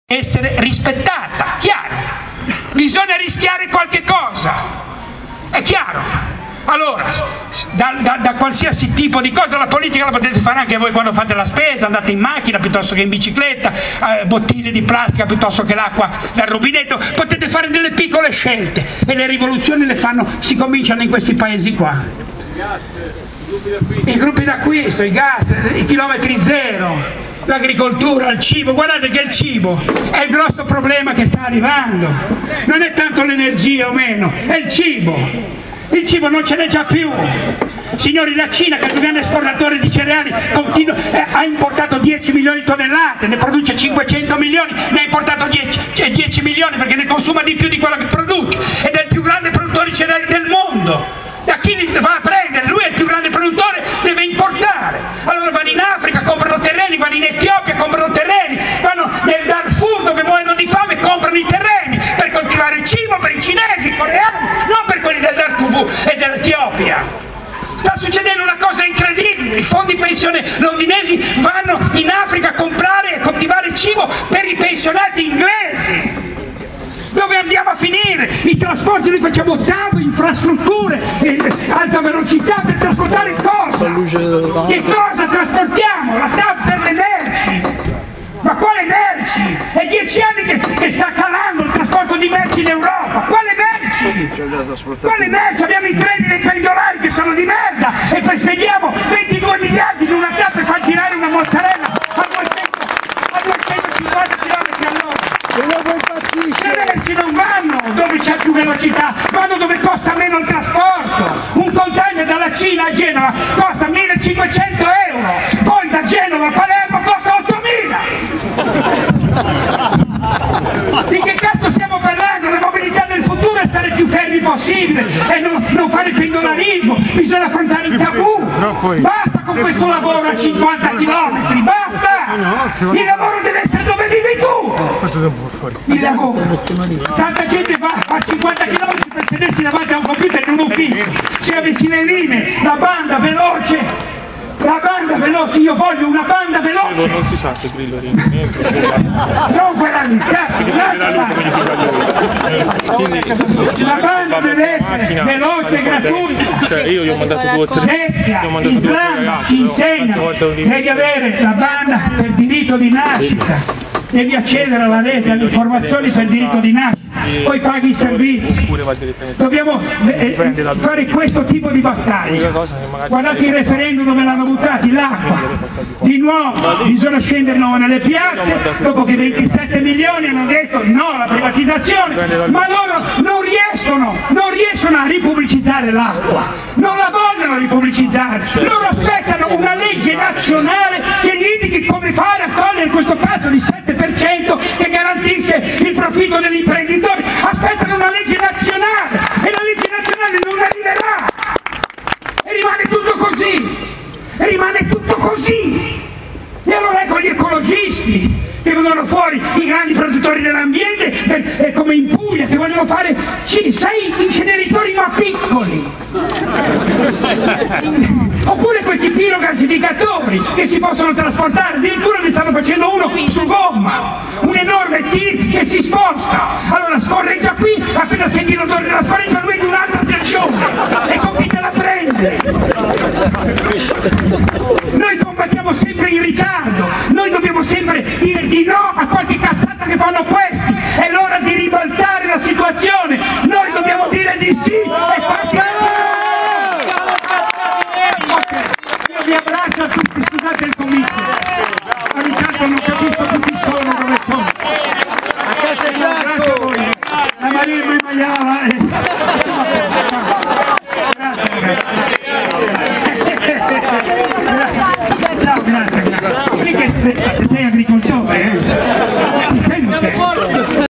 Beppe Grillo, la politica ai cittadini e il lavoro a km0!!
Riprese effettuate dall'Associazione CRETA Pisa, a Fucecchio il giorno 28/11/11

incontro con Paul Connet, Beppe Girllo e Rossano Ercolini, non agli inceneritori e ai pirogassificatori!! l'energia prodotta è inferiore all'energia che si ricaverebbe dal solo riciclaggio (dati di Paul Connet)..bruciare i rifiuti è uno spreco oltre che un danno per la salute dei cittadini che respireranno poi le polveri sottili prodotte!!